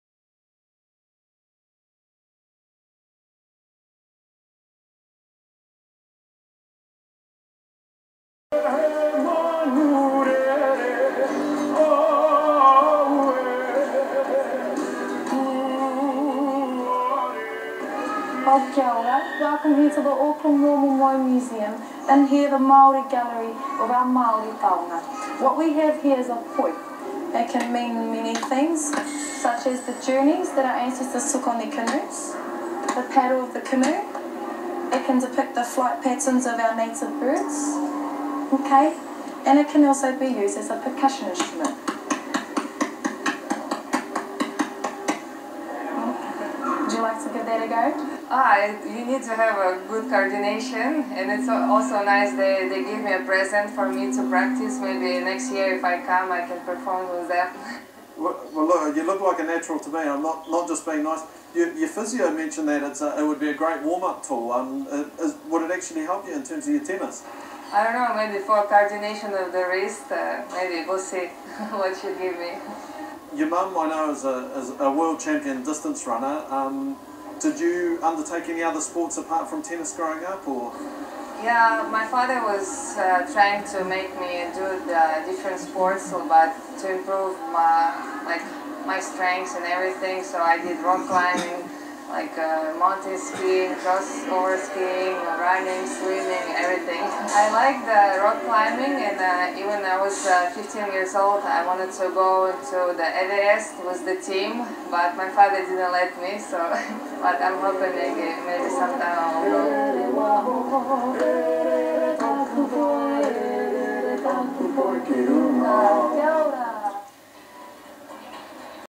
Yaroslava Shvedova sees Maori culture
Yaroslava Shvedova in Auckland experiences some Maori culture.